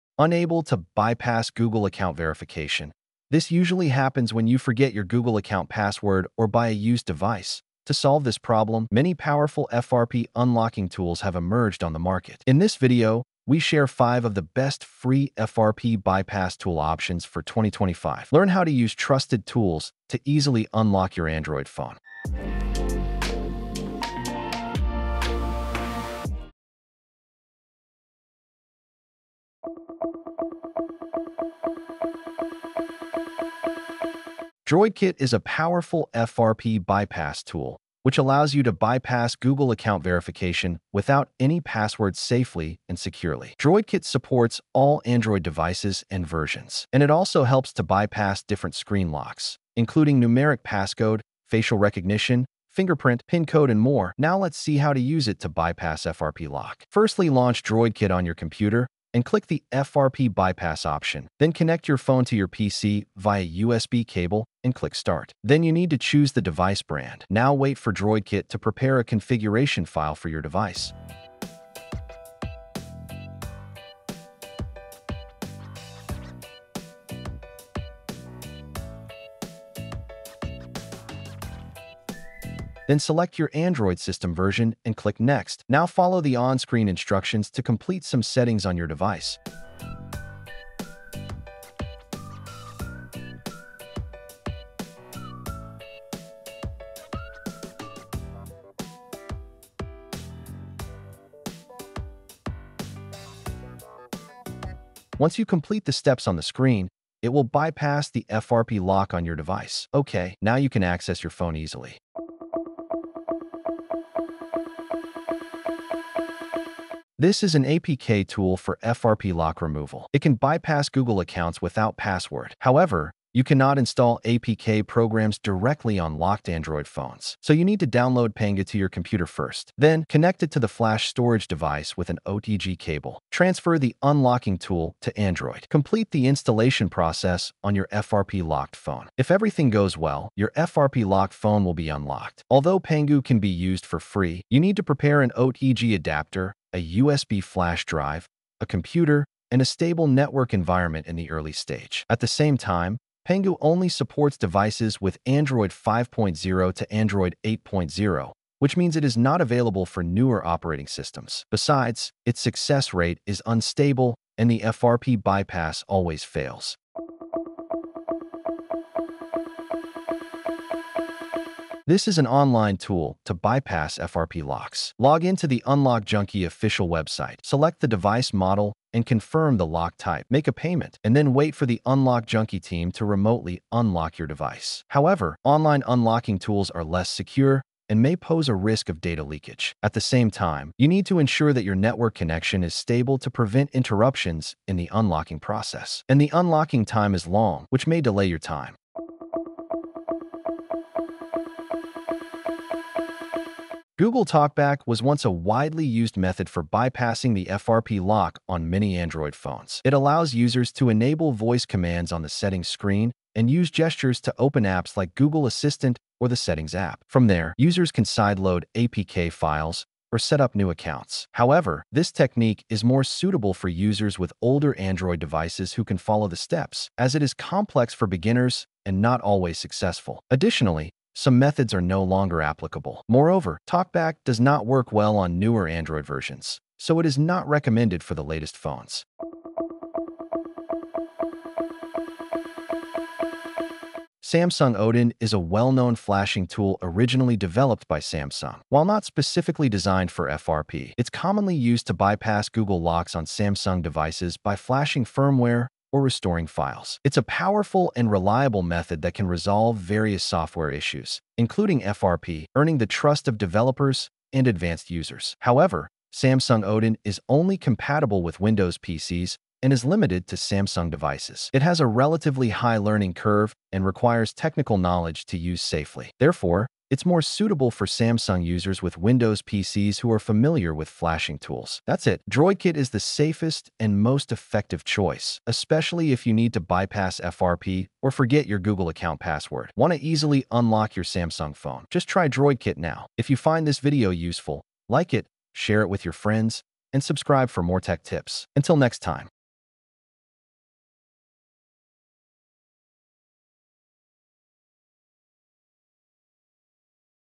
FRP Bypass Tool for PC Free Download | How to Bypass FRP Lock | All Samsung & Android Support
FRP Bypass Tool for PC Free Download? Unable to bypass Google account verification? How to Bypass FRP Lock? 👉 Try iMobie DroidKit - all-in-one Android toolkit here (Free Trial)

This usually happens when you forget your Google account password or buy a used device. To solve this problem, many powerful FRP unlocking tools have emerged on the market. In this video, we share 5 of the best free FRP bypass tool options for 2025. Learn how to use trusted tools to unlock your Android phone easily.
00:00 Intro
00:26 1. iMobie DroidKit [Fast & Easy]
01:01 2. Pangu FRP Bypass APK
02:21 3. UnlockJunky
02:21 4. Google TalkBack
02:21 5. Samsung Odin